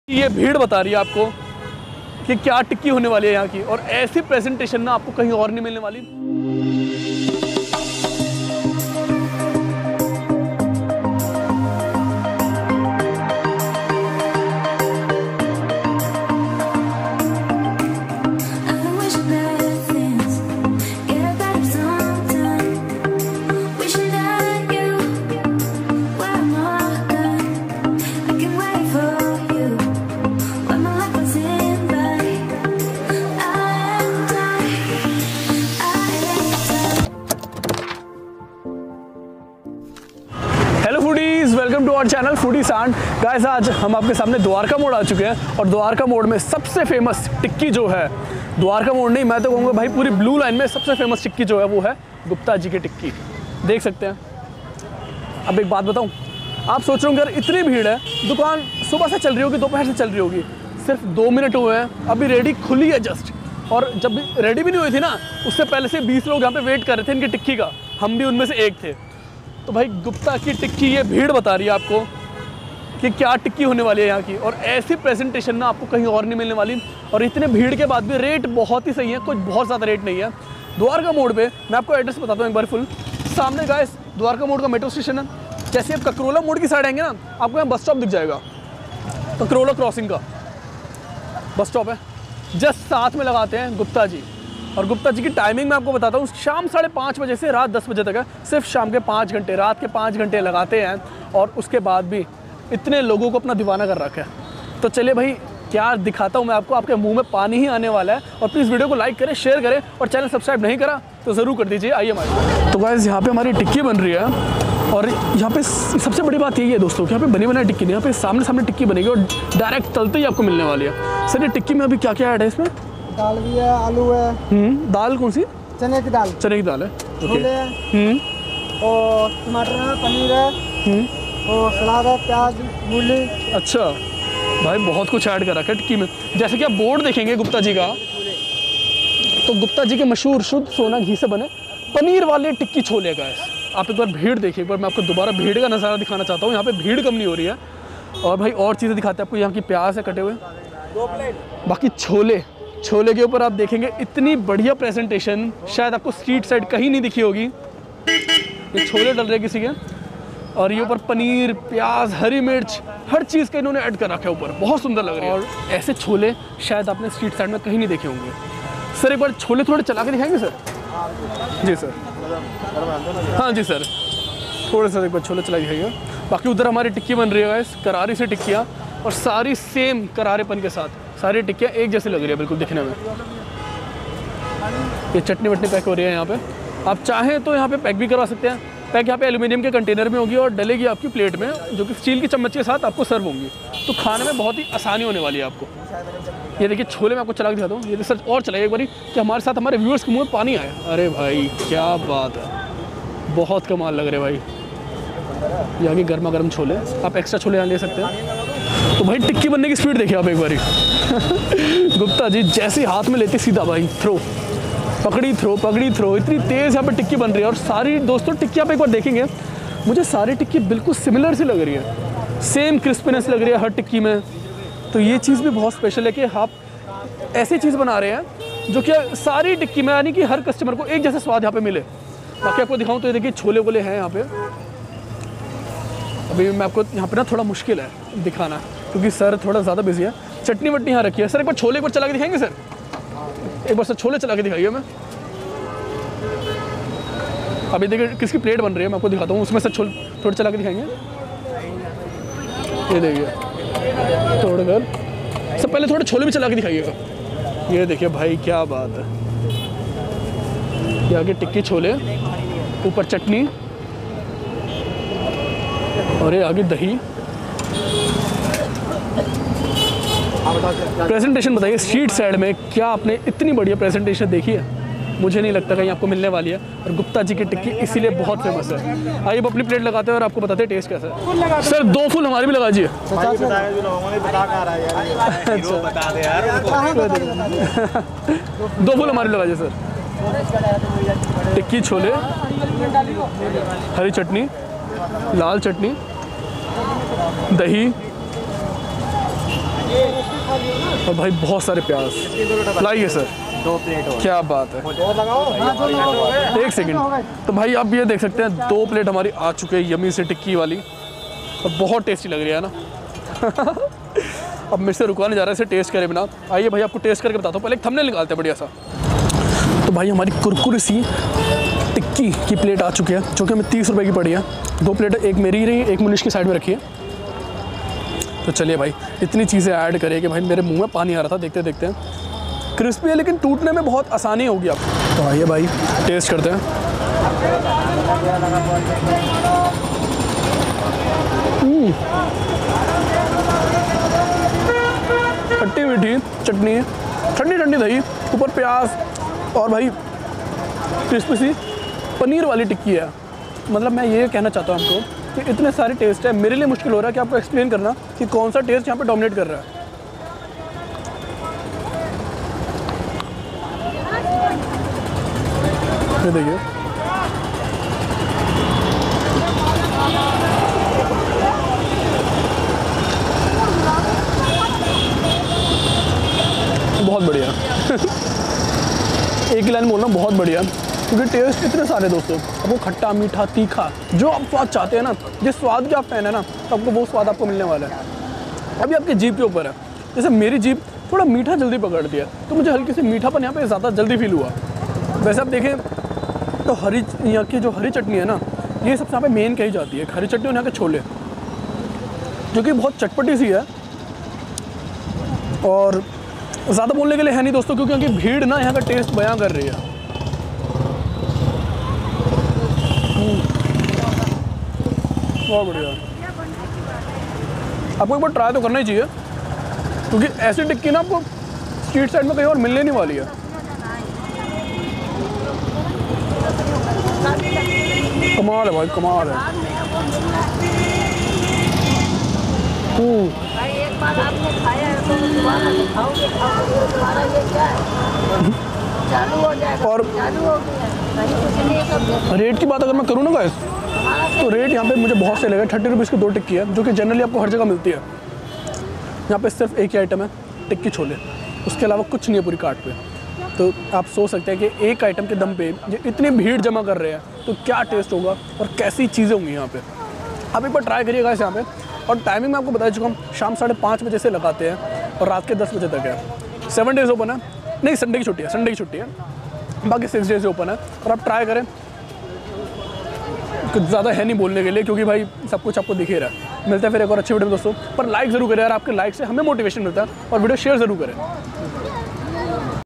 Gupta Ji Ke Paneer Wale Tikki Chole || Delhi Street Food
In today's video we cover "Gupta Ji Tikki Chole" At Kakrola Xing. You Should Definitely Try It .
Shop Address : Near Kakrola Xing Bus Stop, Azad Hind Fauj Marg, Block A, Sewak Park, Kakrola, New Delhi, Delhi 110059
Price : 20/30 Rs Only
Monday to Sunday (No Holiday)
Opening hours - 5:30pm to 10:00pm